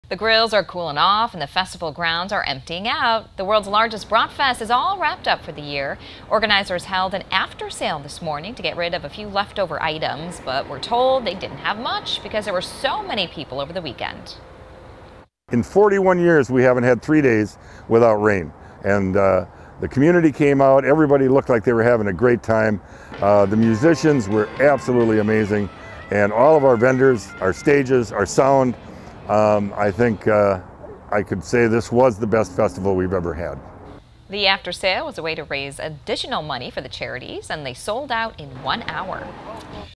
Brat Fest wraps up after a beautiful weekend
One Brat Fest worker said it's the best one they've ever had.